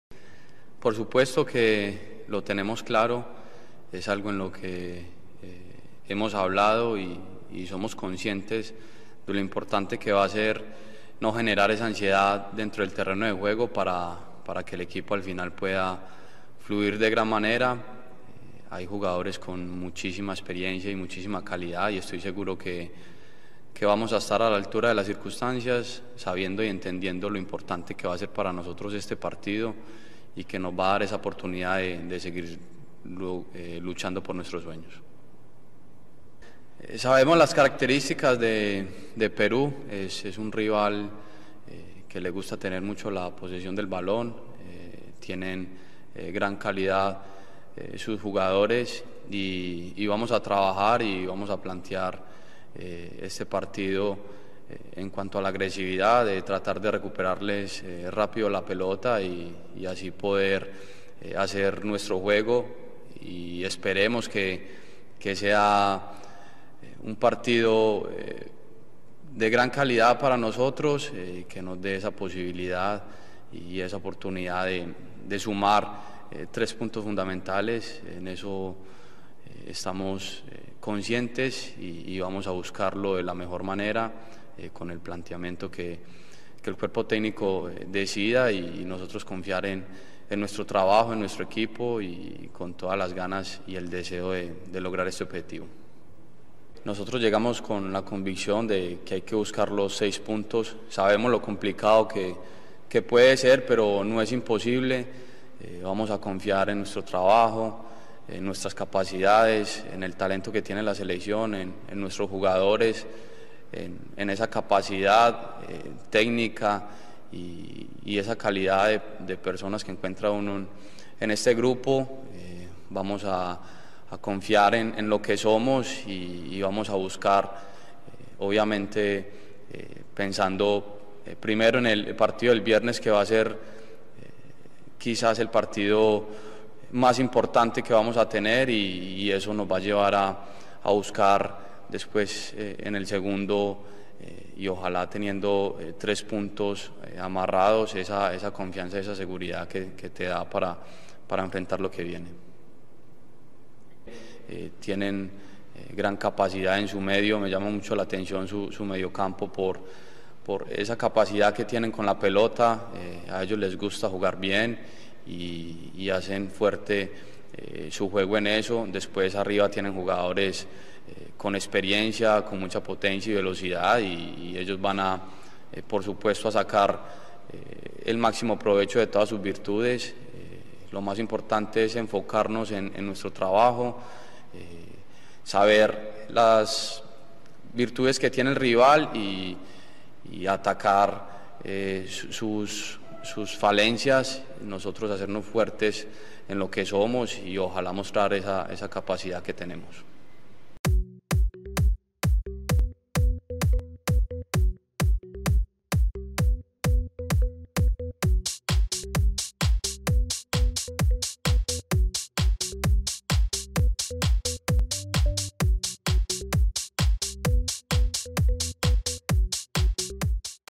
John Stefan Medina OLVIDA su EXPULSIÓN contra Cruz Azul y se ENFOCA en la Selección Colombia
Conferencia de prensa de John Stefan Medina, previo a lo juegos de Colombia contra Perú y Argentina.

🍀 Regístrate en 1xBet con este link y llévate un bono de hasta 130 euros en tu primer depósito